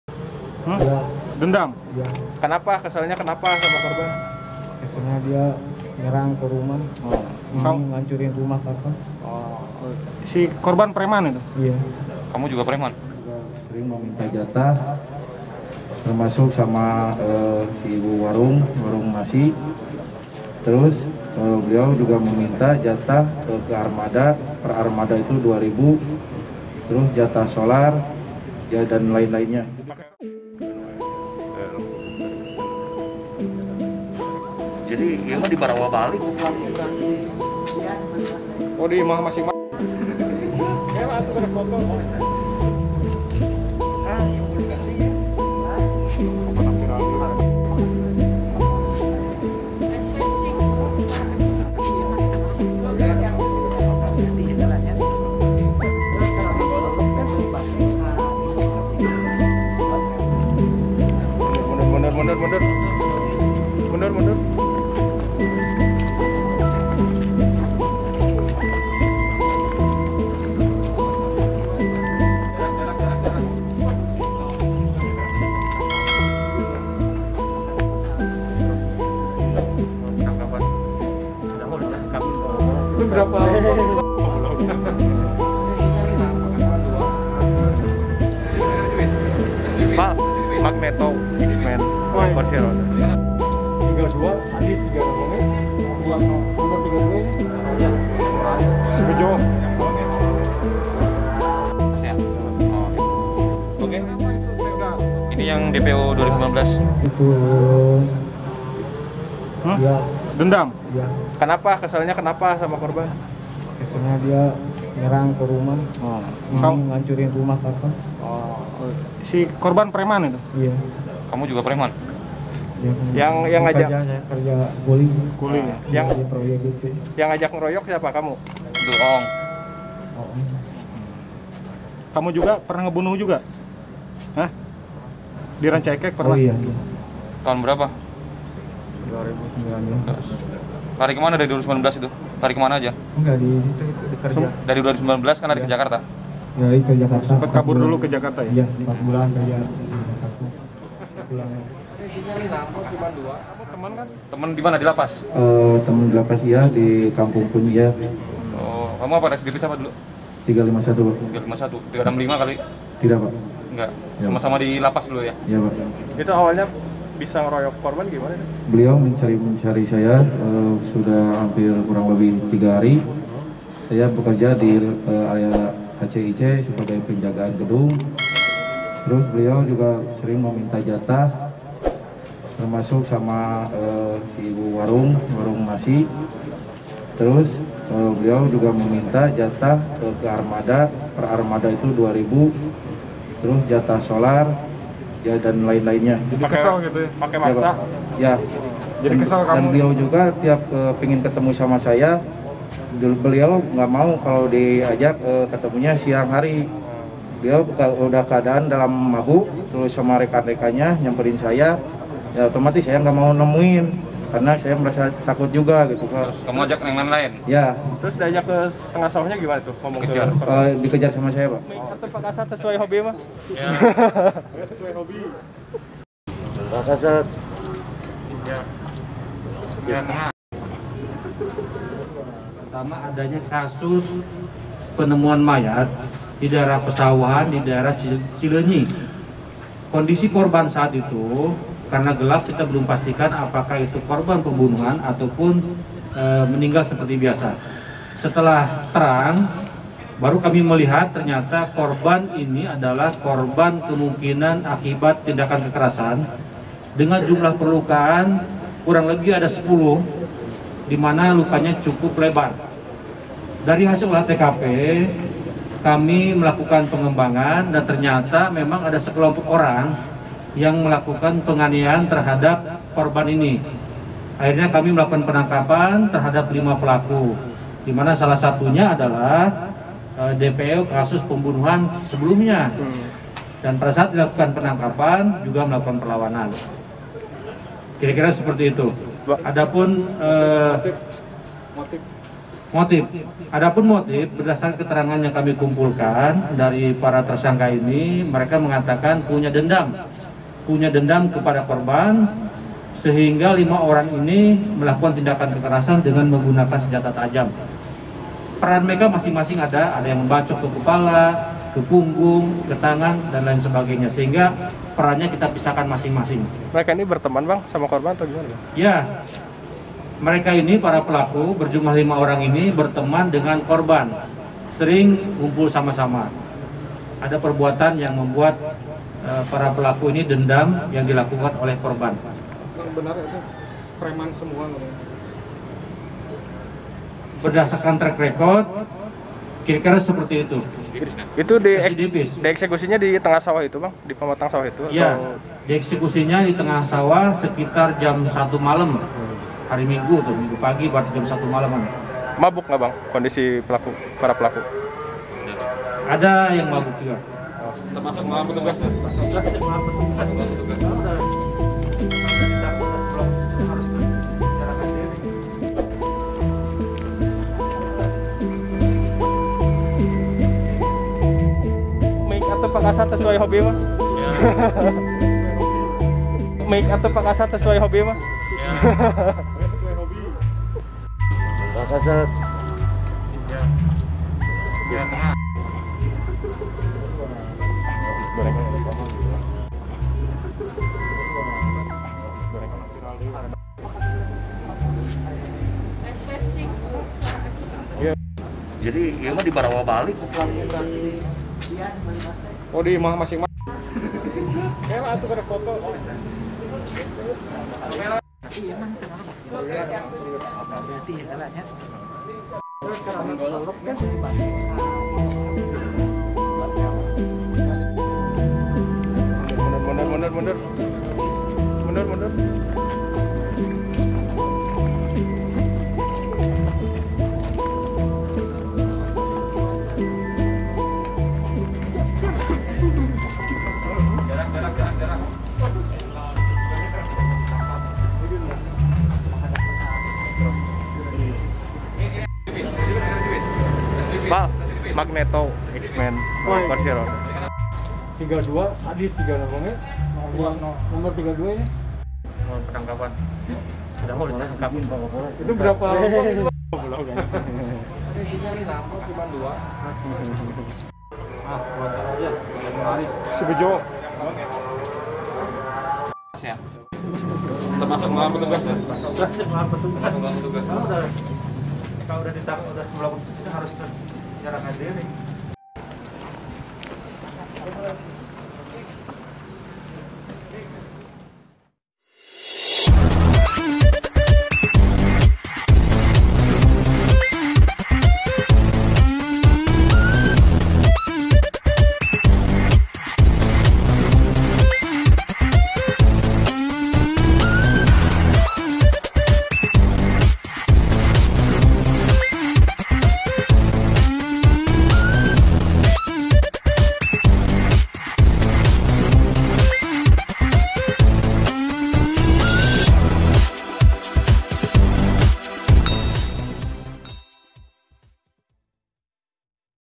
DOOR ‼️POLISI TEMBAK PELAKU PEMBUNUHAAN DIDUGA PREMAN DI CILEUNYI
Polisi berhasil menangkap 5 pelaku pembunuhaan terhadap seorang pria diduga preman di Kecamatan Cileunyi, Kabupaten Bandung. Satu dari empat tersangka ditembak polisi karena melakukan perlawanan saat dilakukan penangkapan.